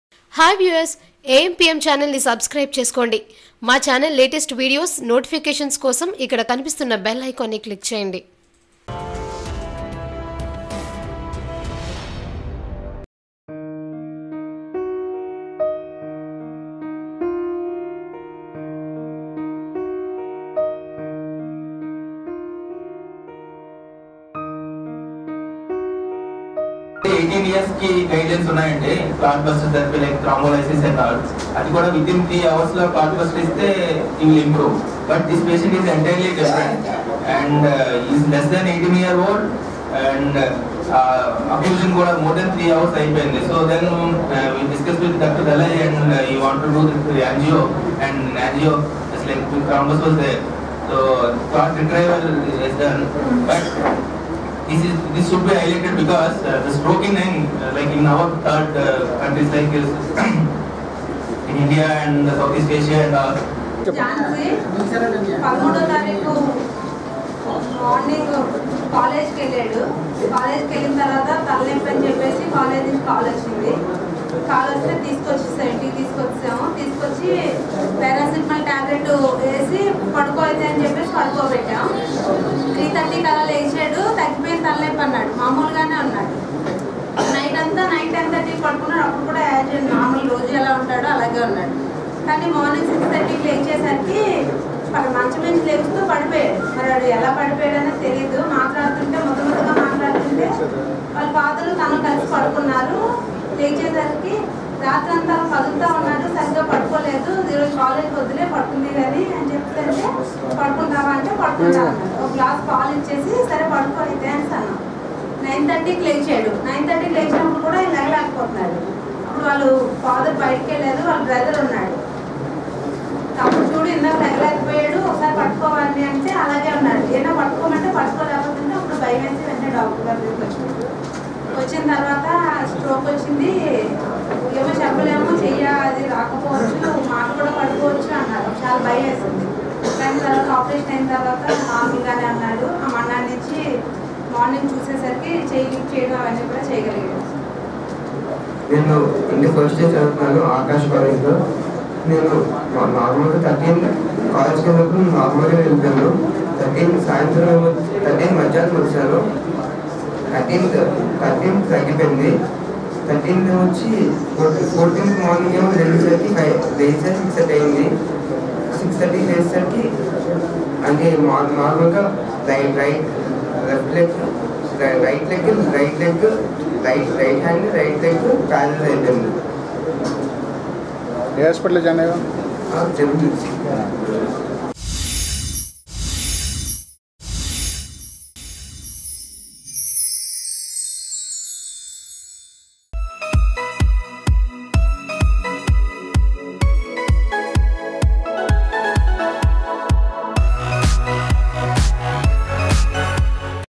24 Hours Service in Seven Hills Hospital for Brain Stroke Patients
AMPM Live  : 24 Hours Service in Seven Hills Hospital for Brain Stroke Patients